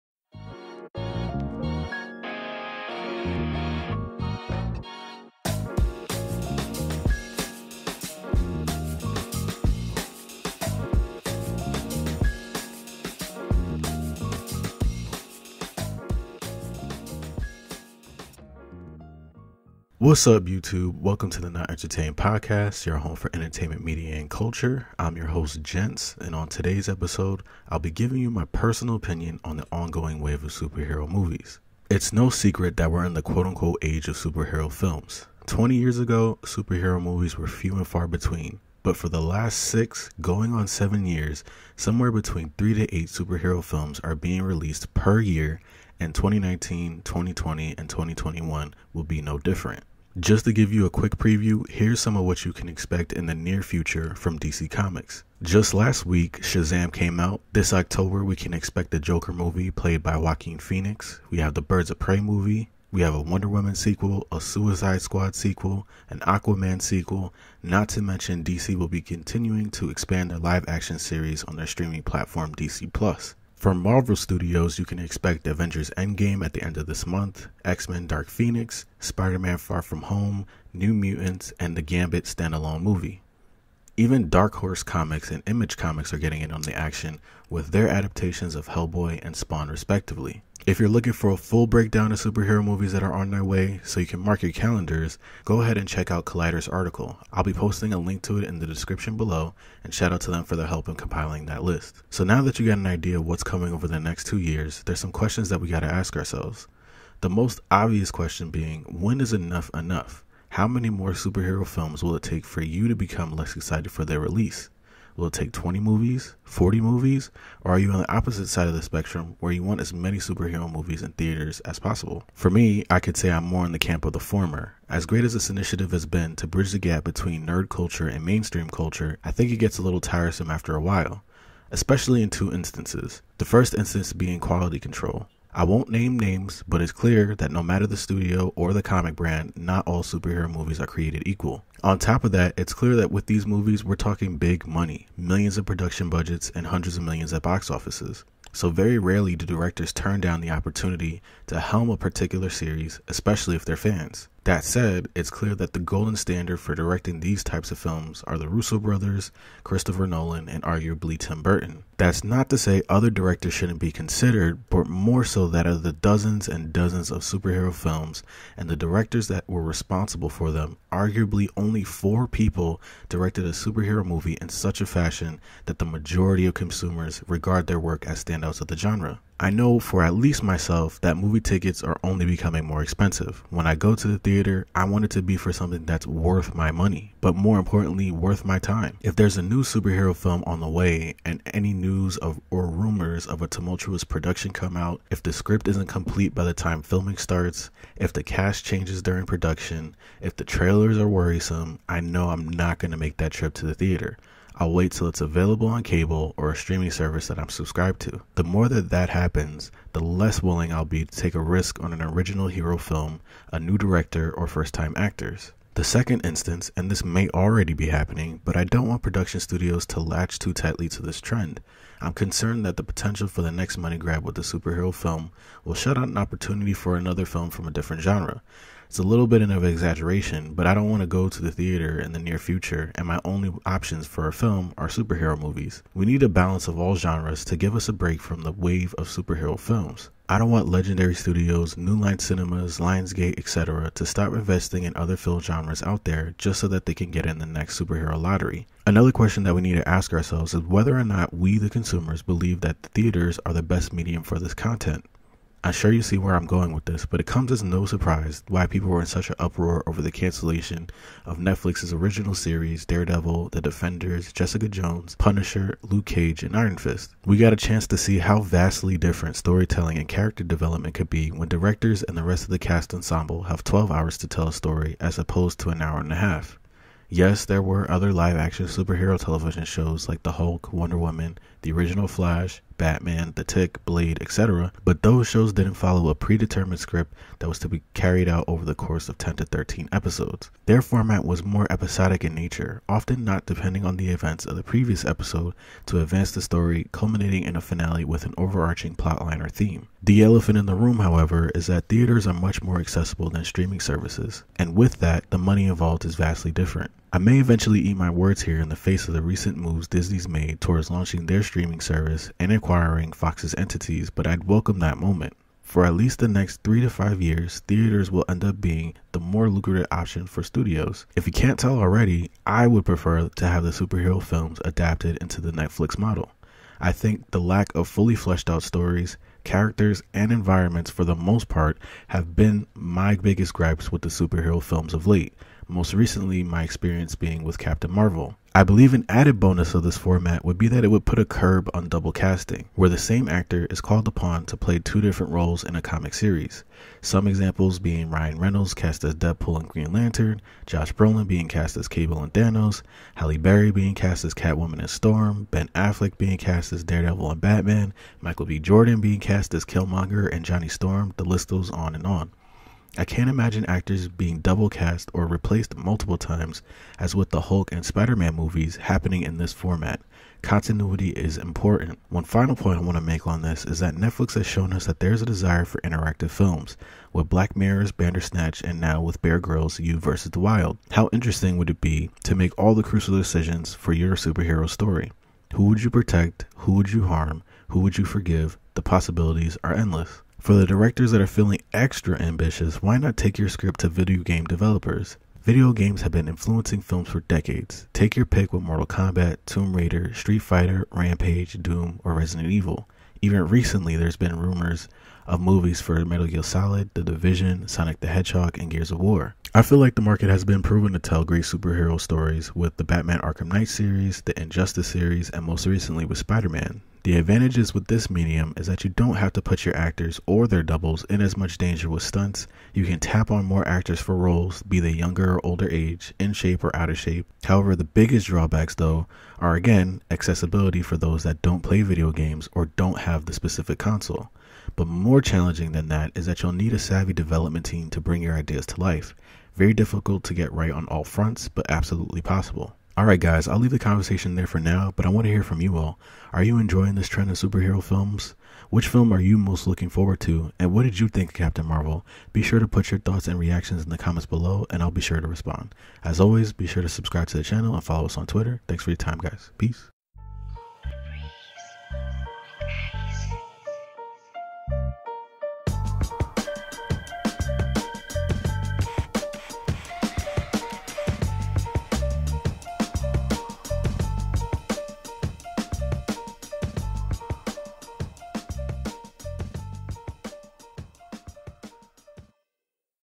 KE Podcast Ep. 6: Superhero Films
Welcome to the Knot Entertained Podcast, your home for entertainment media and culture. On this episode, Gentz gives his opinion on the superhero film trend.